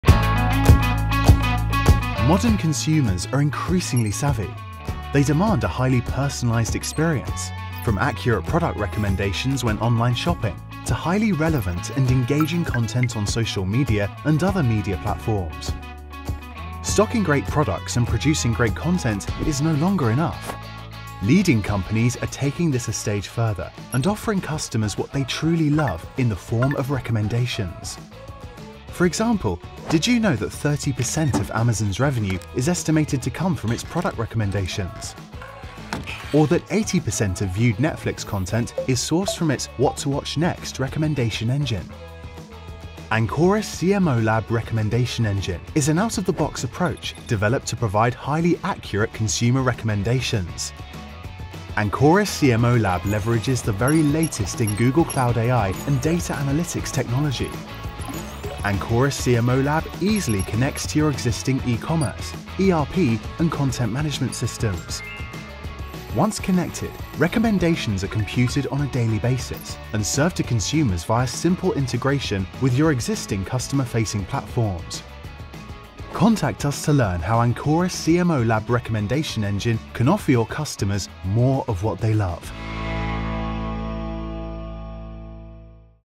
Offer customers more of what they love with recommendation engines
Ancoris CMOLab Recommendation Engine leverages leading-edge AI from Google Cloud to provide customers with a highly personalised experience. From recommending products and services, to offering highly-engaging and relevant media content,  a recommendation engine is proven to increase revenue, customer retention and customer engagement.